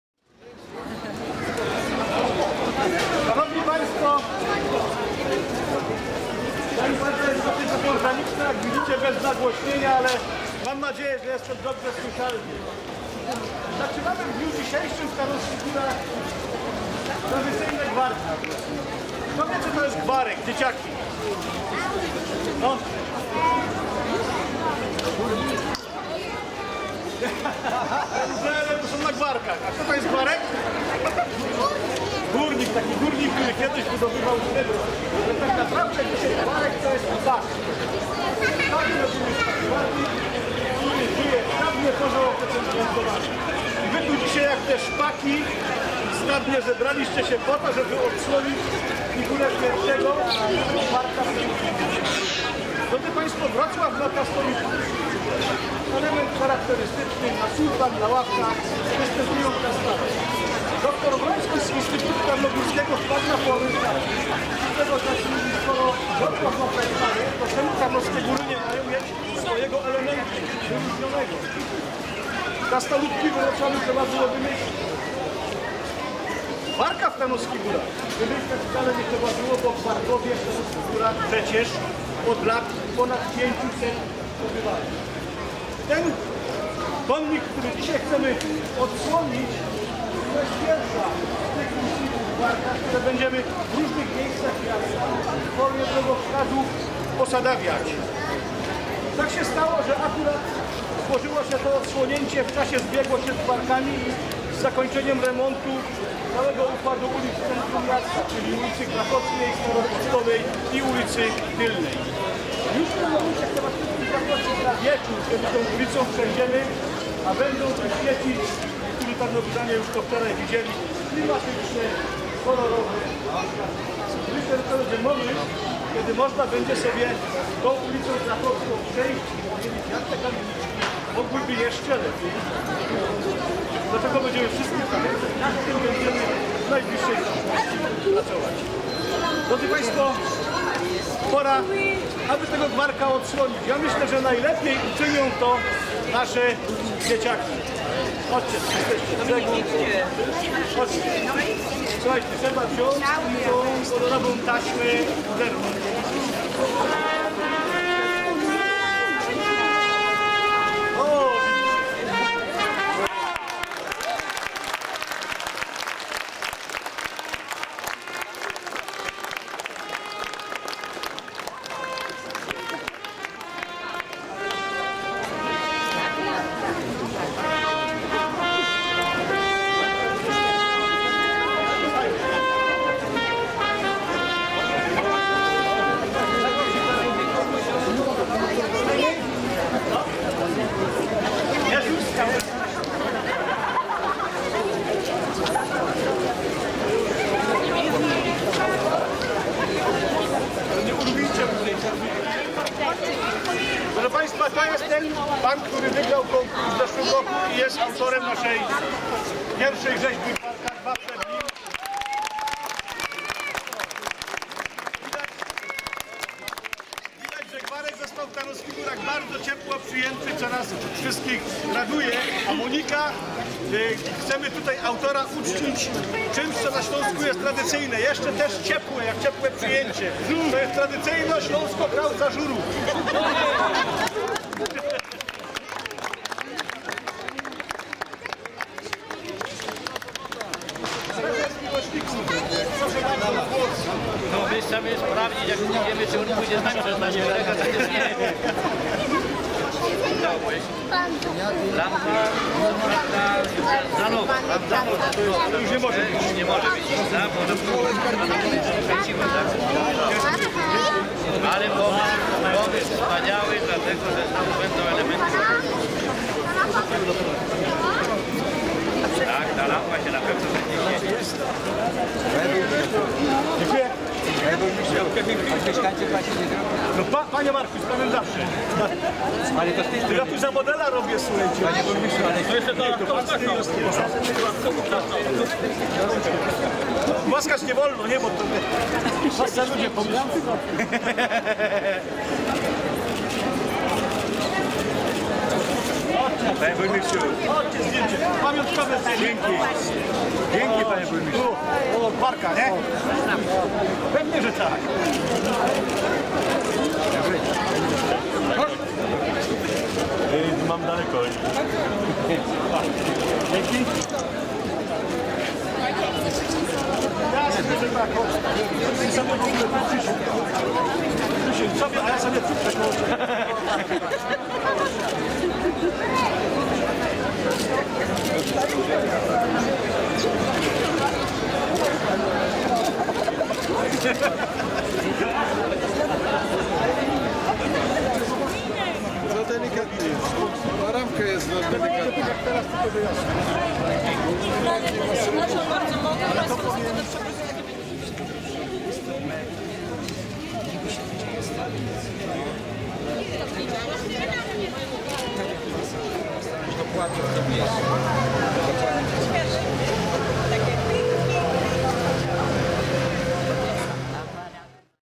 Gwarki 2013 - Uroczyste odsłonięcie rzeźby Gwarka
Uroczyste odsłonięcie rzeźby Gwarka u zbiegu ulic Krakowskiej i Tylnej
Tarnowskie Góry - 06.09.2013 - Gwarki 2013